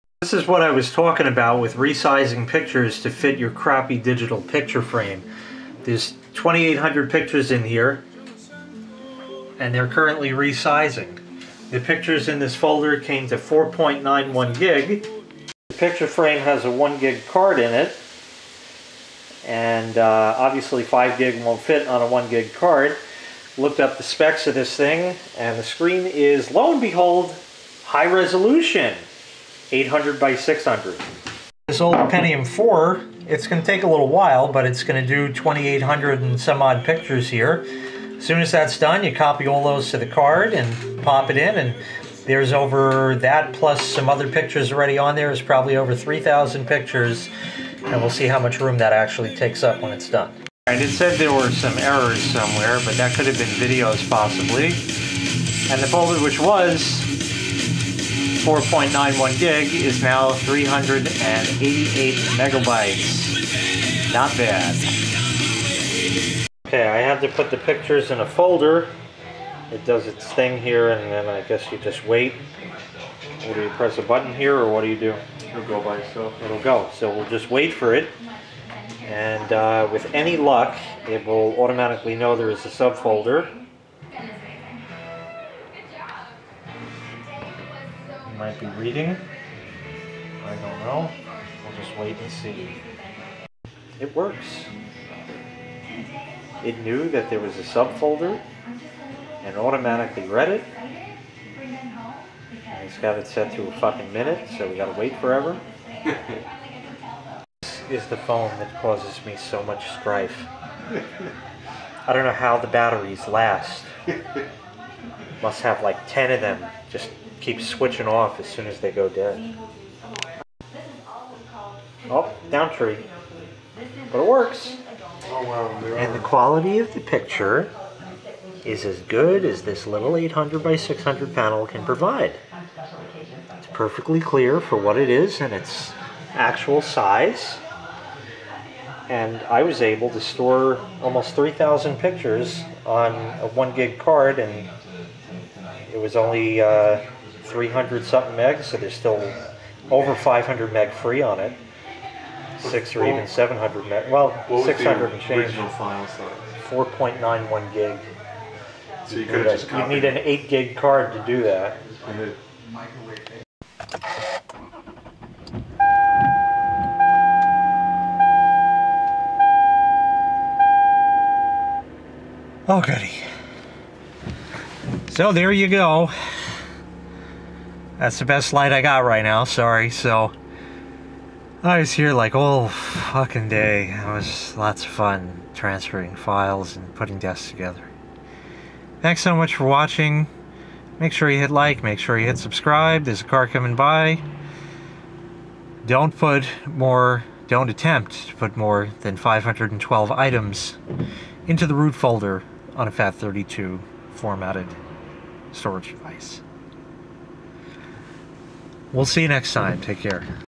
Resizing Digital Pictures To Fit Your Digital Picture Frame
Originally recorded February 24, 2016.

A while ago, xjoe81x had lent me a 2GB SD card.  Now he wants it back.  I haven't had a chance to test out what I needed with said card yet.  He told me that he has a lot of pictures he wants to put on his digital picture frame, but they won't fit on the 1GB card that he has in it.

I looked up the specs of the digital picture frame, and it claims it's high resolution.  That was to say 800x600 resolution, not high by any standards today.  Being that most of the photos are multiple-megapixel, and several megabytes in size, there's a fair amount of resizing the picture frame has to do on the fly.  That's not necessarily a problem, as it was designed for it.

But if the pictures are resized first to 800x600 resolution, they will be a lot smaller in size on the SD card.  So I took all the pictures on the card, plus all the other pictures he wanted to add, and resized them to the panel's native resolution of 800x600.  I was able to now fit 5GB worth of pictures onto a 1GB card, with hundreds of megabytes to spare, with no real degradation in picture quality.  (Of course the original pictures were left intact for other purposes, I had made a copy of the folder containing all the pictures and resized those).